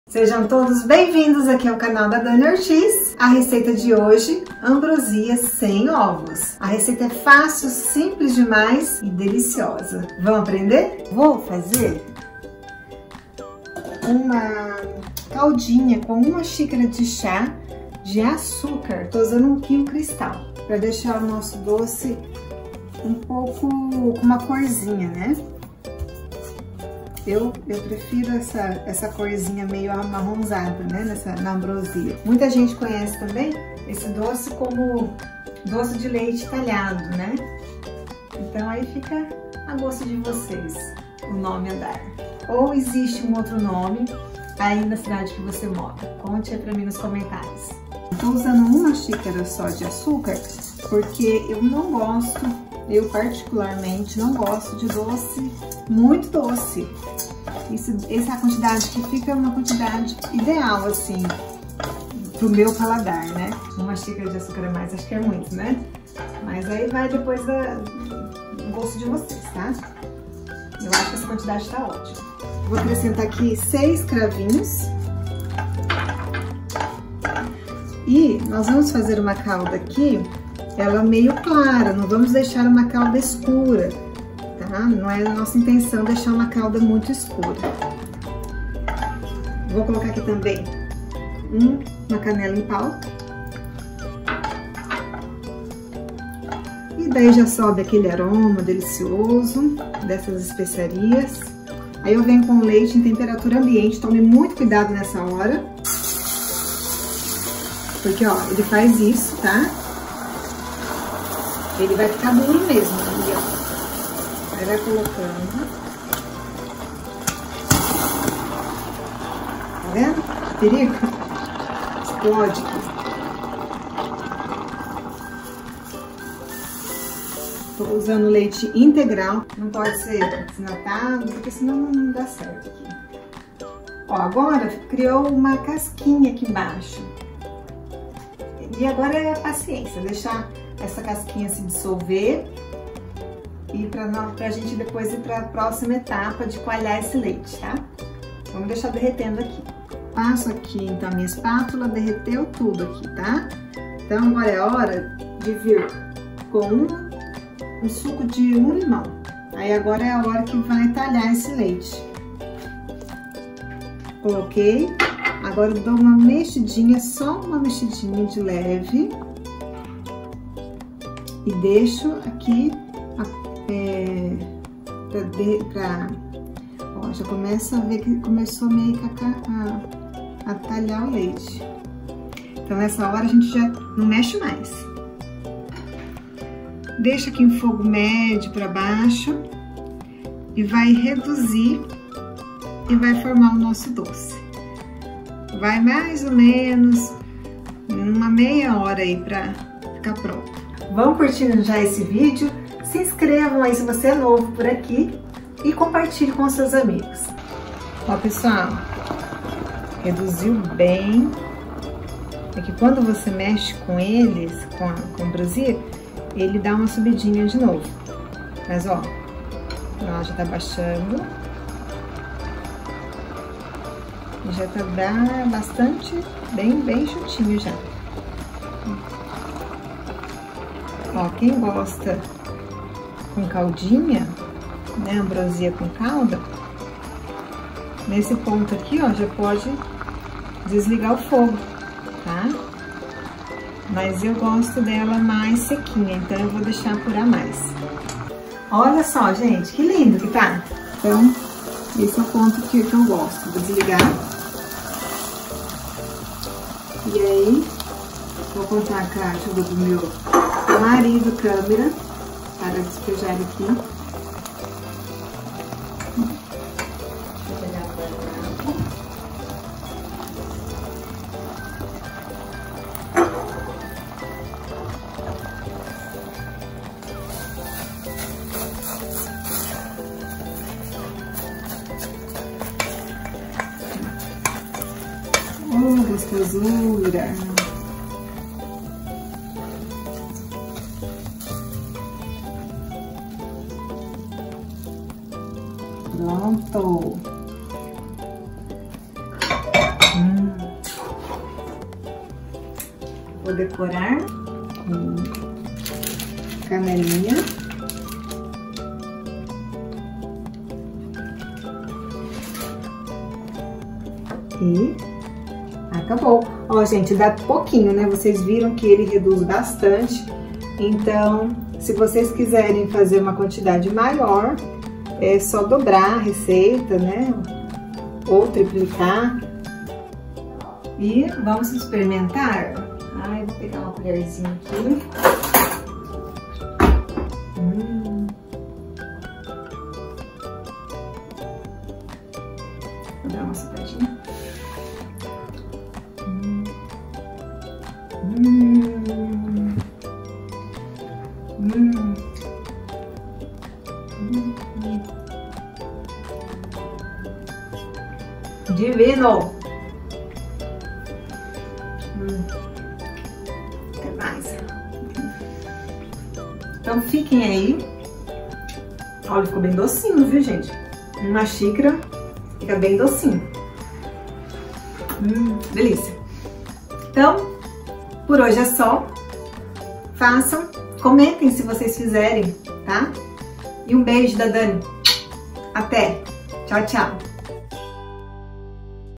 AMBROSIA | SEM OVOS!
COMO FAZER AMBROSIA | SEM OVOS!

Receita
- 1 xícara (chá) de açúcar
- 6 cravos da índia
- 1 canela em pau
- 1 litro de leite integral
- suco de 1/2 a 1 limão (dependendo tamanho do limão)